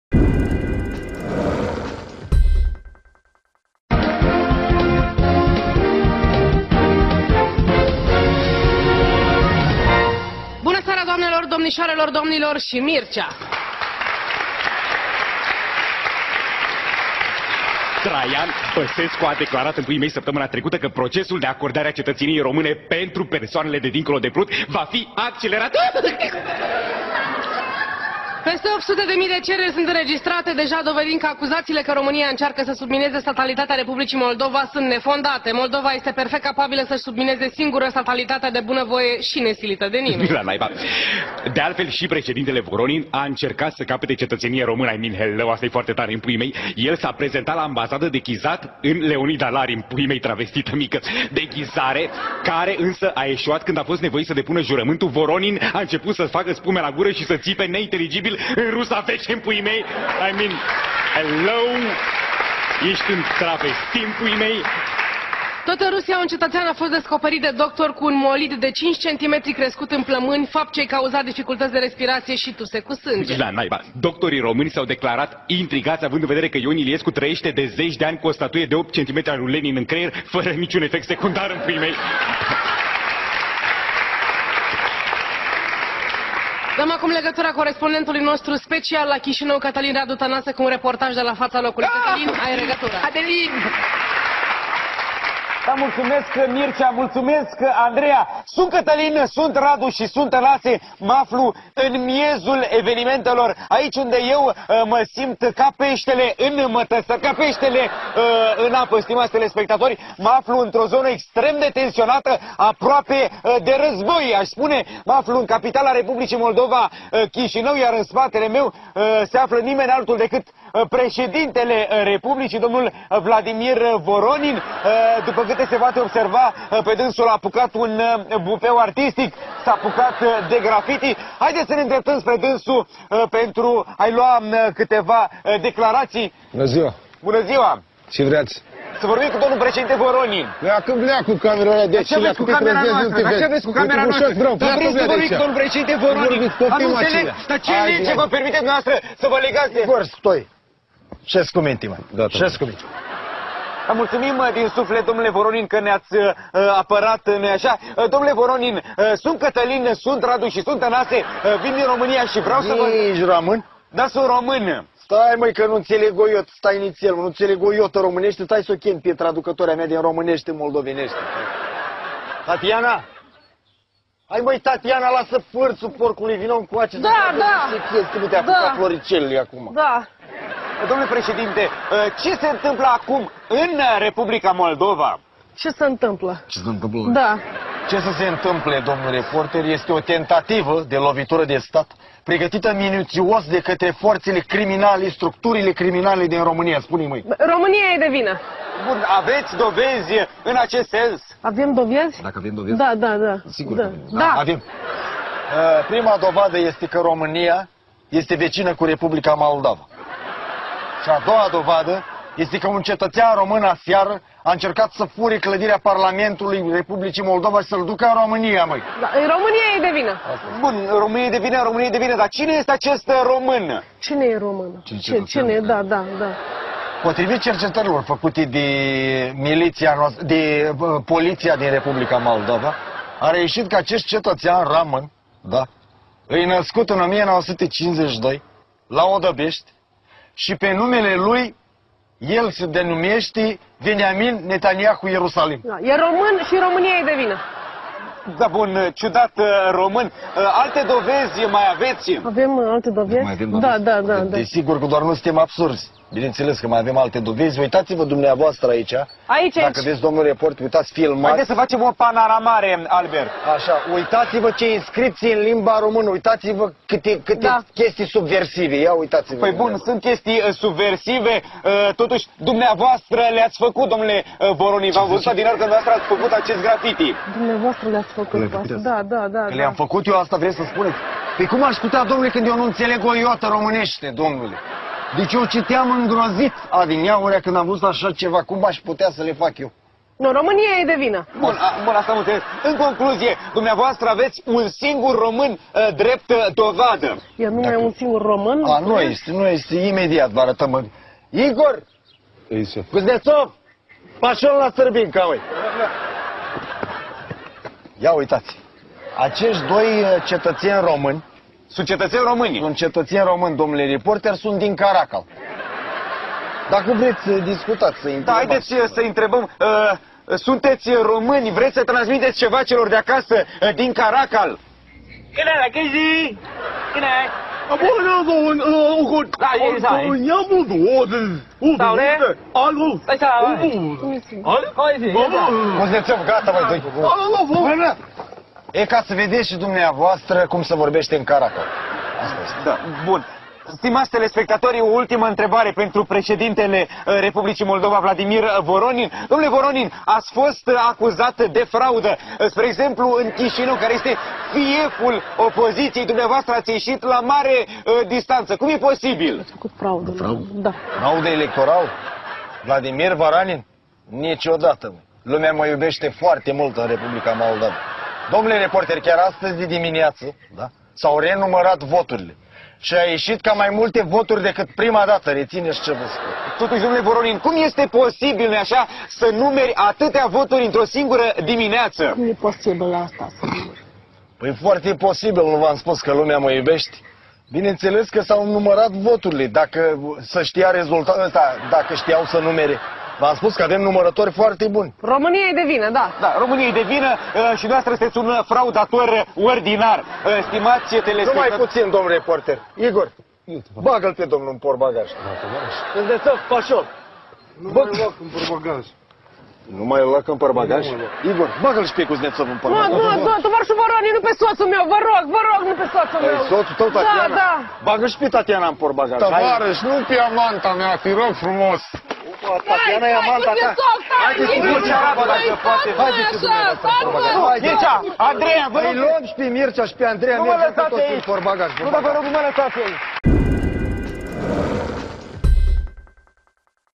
ROMANIA E DE VINA-SUPER FILMULET
UN FILMULET FOARTE COMIC SI TARE, CARE DEFAIMEAZA IMAGINEA LUI VORONIN